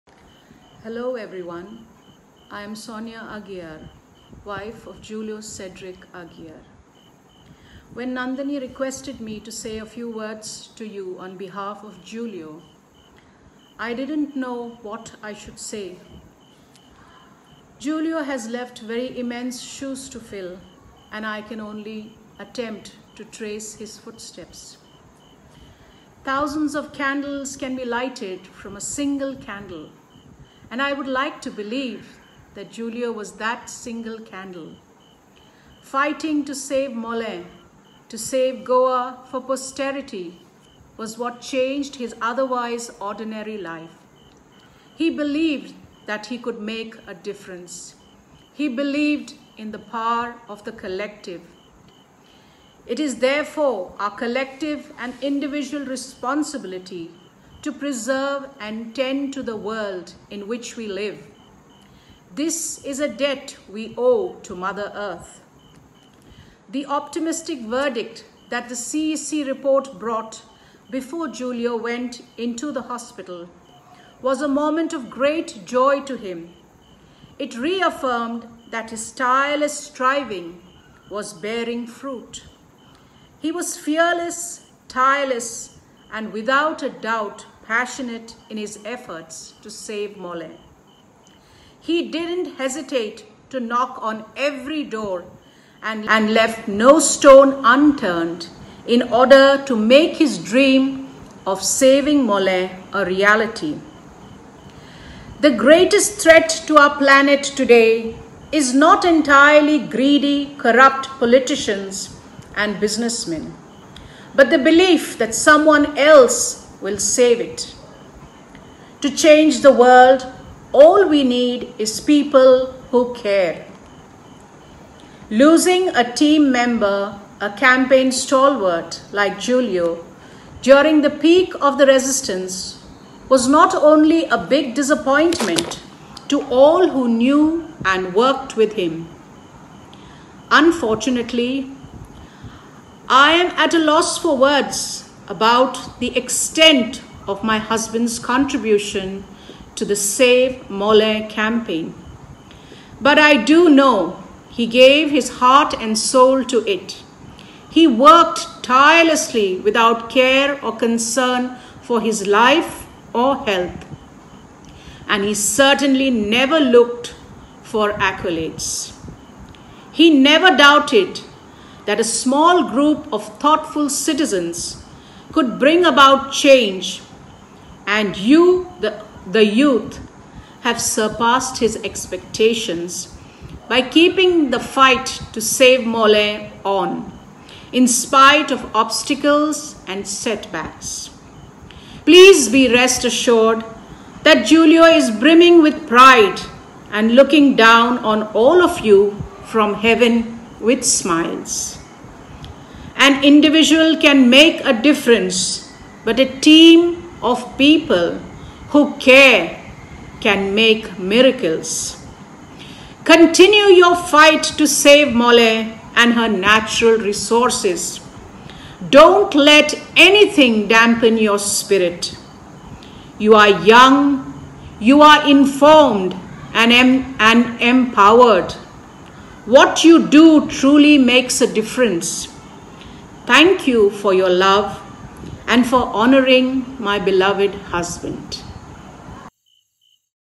A heartwarming message from Sonia Aguiar on our beloved Julio's Aguiar's birthday [7th February, 22]
Today we celebrate a beautiful soul, Julio Aguiar - an inspiration to the youth of Goa and a guiding light whose spirit inspires every Amche Mollem volunteer. Sonia Aguiar, W/O Julio Aguiar pens some heartwarming words for our beloved Julio.

Julio was tireless in his commitment to safeguarding the forest, and his persevering spirit inspired many of us to be more present in the time we gave to Amche Mollem.

When we say he did it all, we mean it! He was an organiser that built a community for the forests of Mollem. Our public spaces were lit up in vibrant flash mobs, songs, banners, competitions, press conferences, countless field visits and petitions - all through Julio's efforts.

Julio Cedric was a true son of the soil. Present and future generations owe so many of the Amche Mollem successes to him. We will always remember him, and know that his beautiful spirit will continue to instruct our efforts to keep vigil over our forest and our future.

We will carry you along this journey to safeguard our Goa, always inspired by your spirit and heart.